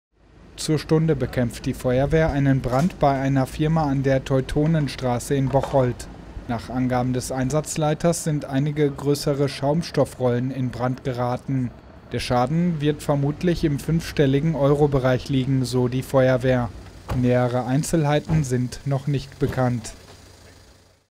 Brand bei Firma in Bocholt
Bei einer Firma an der Teutonenstraße ist in der Nacht ein Feuer ausgebrochen.